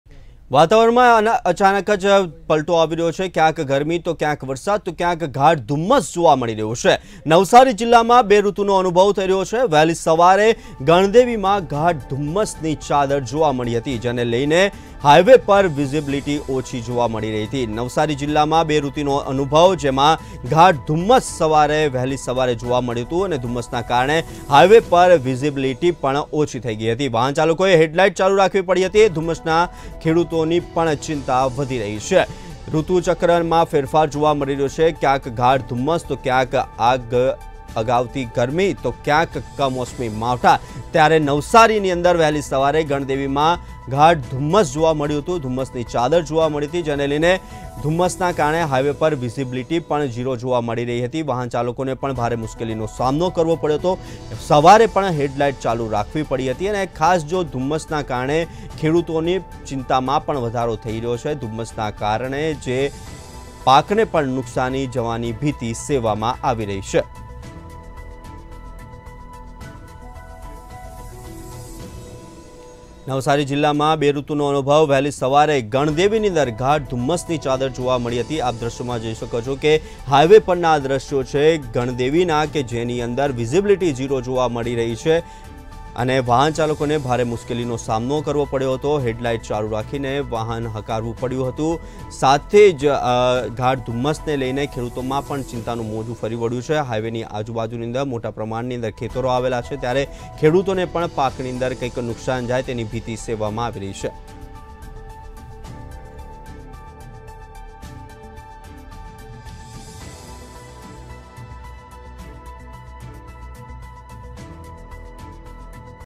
Navsari જિલ્લામાં બે ઋતુનો અનુભવ, વહેલી સવારે ગણદેવીમાં ગાઢ ધુમ્મસની ચાદર | MantavyaNews
Experience of two seasons in Navsari district, heavy fog cover in Gandevi early morning